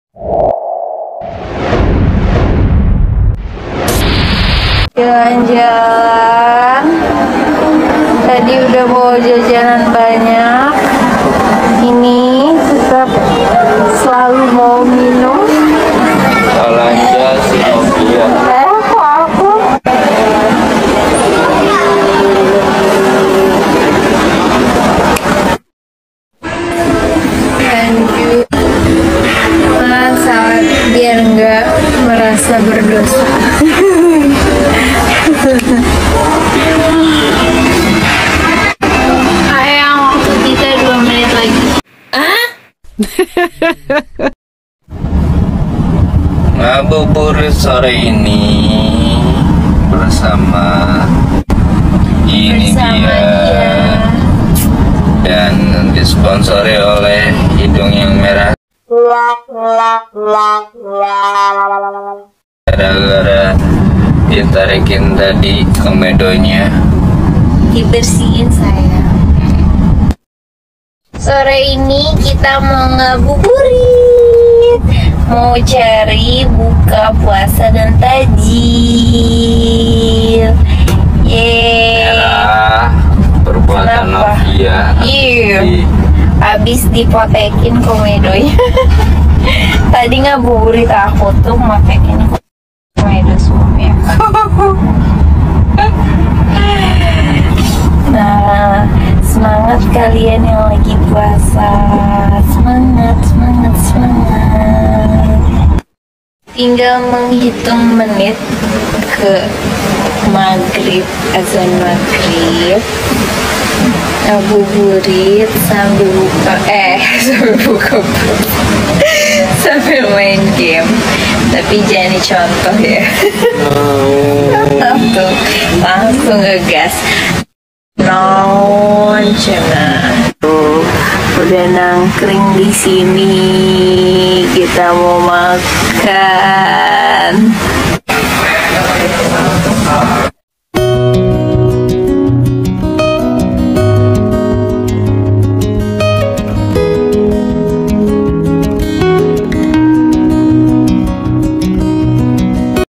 Ngabuburit Hari Ke 3 Puasa Ramadhan Ikbal Fauzi dan Novia Giana
Assalammualaikum

Ngabuburit hari ke 3 puasa Ramadhan ala pasangan Ikbal Fauzi dan Novia Giana. Artis Ikbal Fauzi pemeran Rendy di sinetron Ikatan Cinta dan istrinya, Novia Giana Nurjanah, ngabuburit di hari ke 3 puasa ramadhan, Ikbal Fauzi menunggu waktu berbuka dengan mengelus-elus perut Novia Giana yang saat ini tengah mengandung anak pertama.

Wassalammualaikum
Zafirah Sheza

Terimakasih.